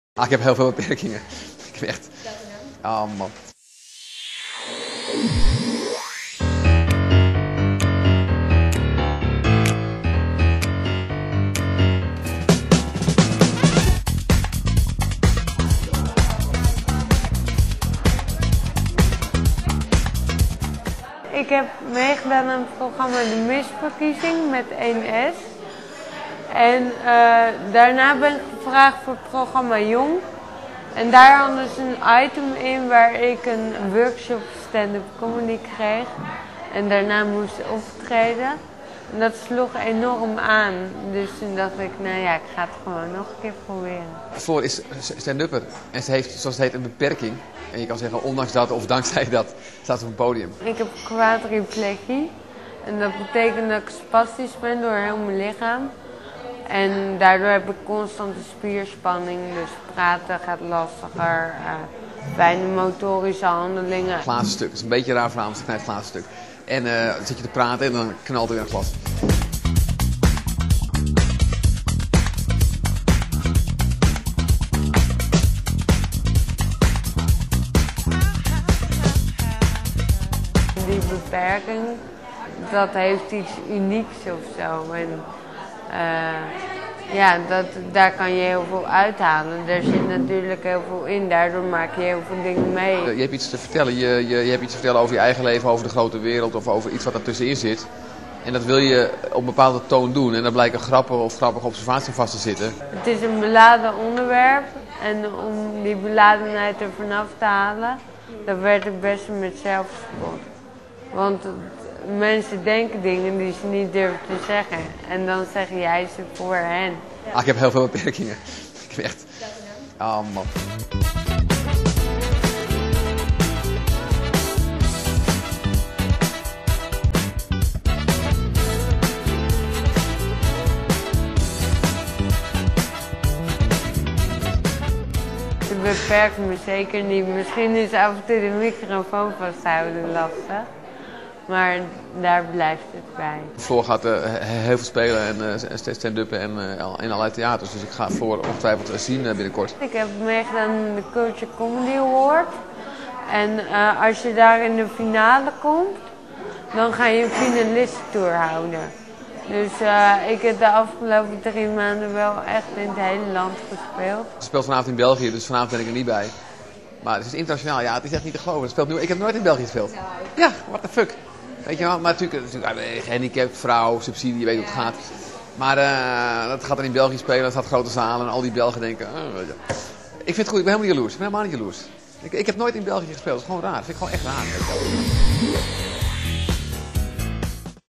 Dolf Jansen interviewt Floor van der Wal - studente met functiebeperking - 2008
Dolf Jansen interviewt Floor van der Wal - studente met functiebeperking voor CampusTV, HvA